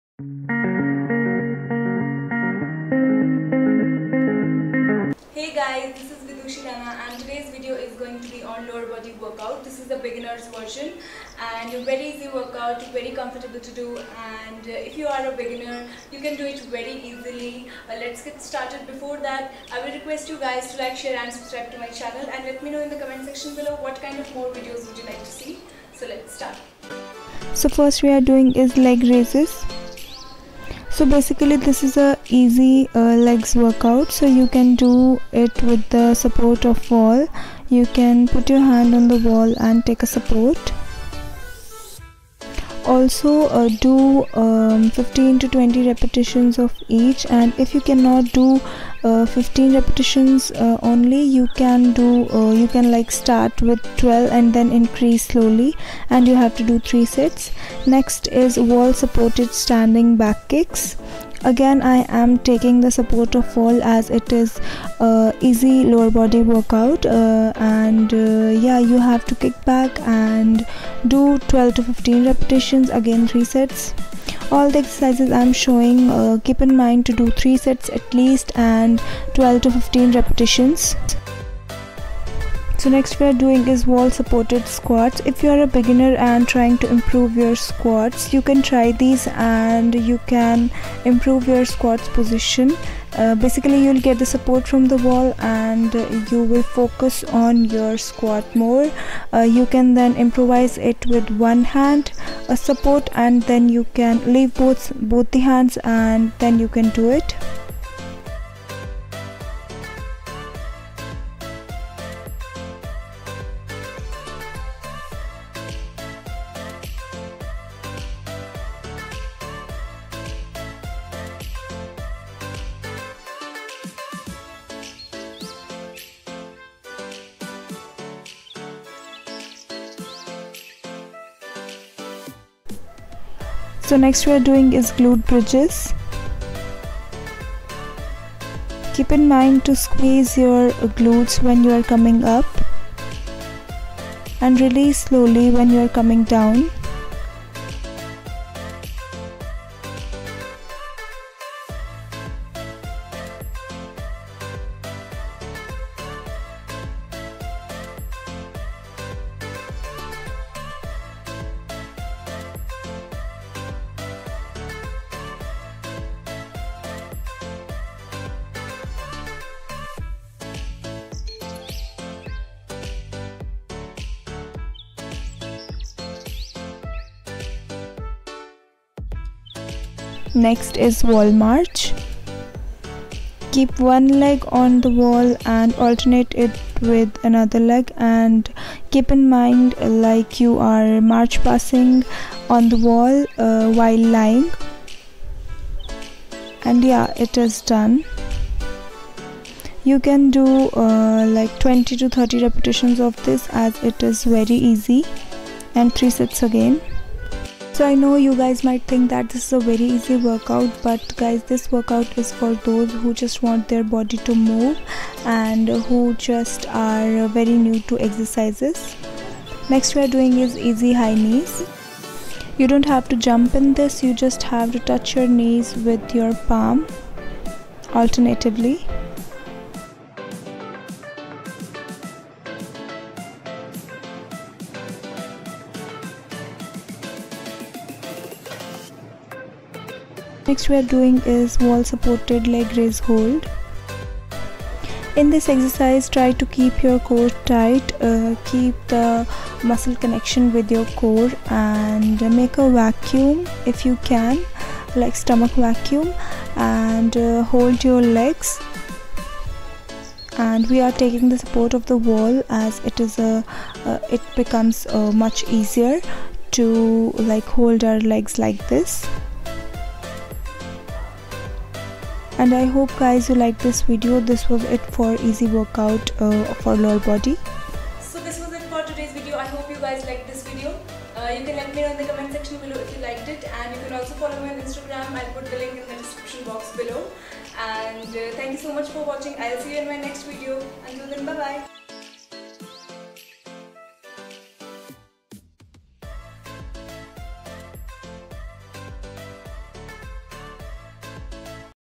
Easy Legs Workout 💮Beginner's Lower Body Workout✨ Home Workout Series Ep. 6💞By Vidushi Raina
I have started a home workout series and I upload home workouts every Wednesday. I hope you guys will enjoy and learn how you can do different kinds of workouts at home with no extra equipments. This video is about lower body Workout💪✨

👉 Keep the mind and muscle connection in each workout.

👉 Try to do these workouts with proper technique.

👉 If you have any physical problems or pain please consult your doctor before doing these exercises.